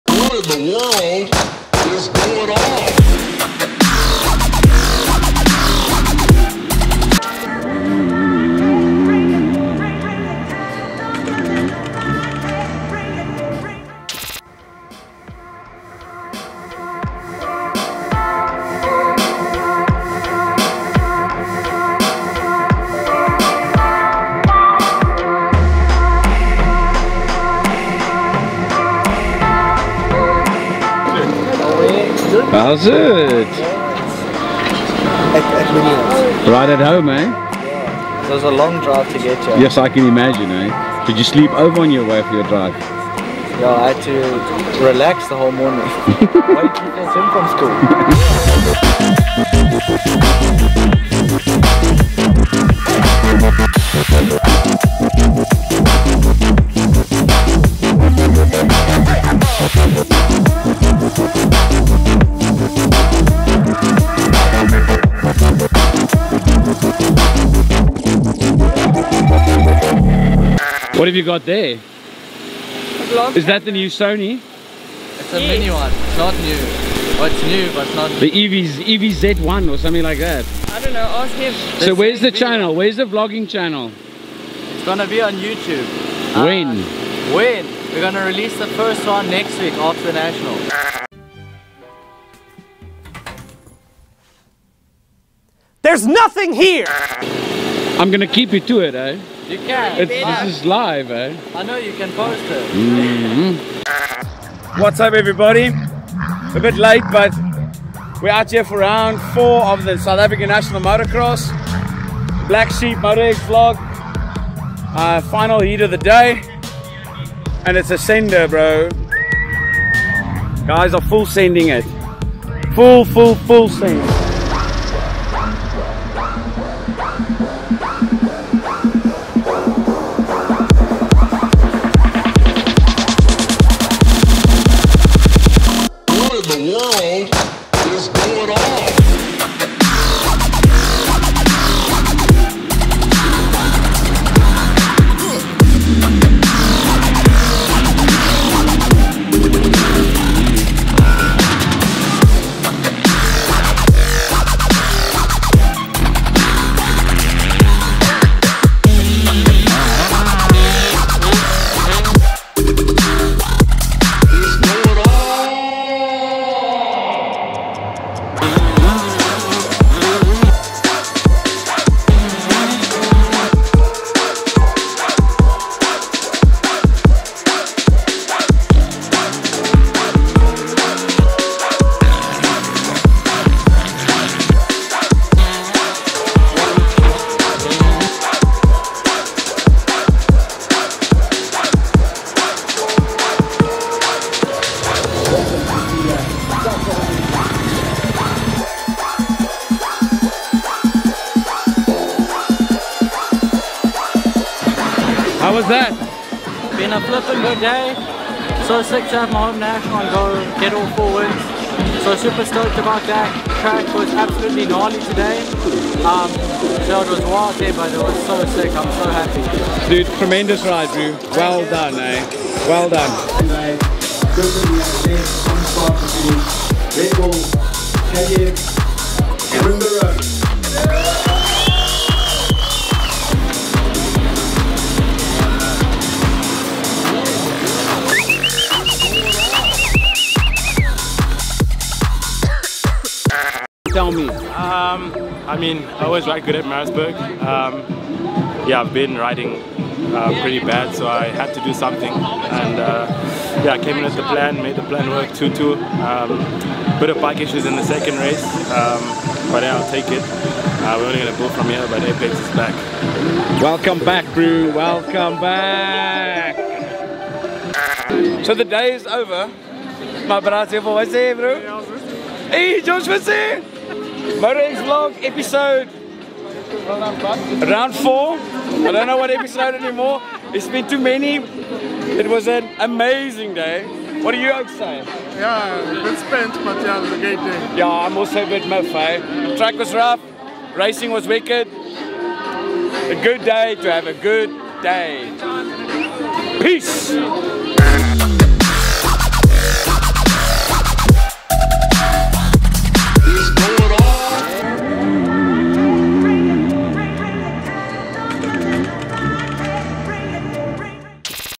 Moto X Vlog - Revving it UP at the South African National Motocross!
Get ready to ignite your passion for motocross as Moto X Vlog presents "Revving it UP at Motocross Nationals!" Join us as we dive headfirst into the heart-pounding action and exhilarating thrills of the Motocross National Racing!

Defending MX2 Champion Cameron Durow a local Pietermaritzburg racer and current National Championship leader in both the MX1 and MX 2 had an amazing race day, taking home 4 wins on the day, what a performance! waiting for the vlog Cameron. 🤣🤣🤣

Jono Mlimi from @mlimiracingco  has a Chat with us after the heat races, and it seems Apex is Back, after claiming a solid P2 for the day! well done Bru!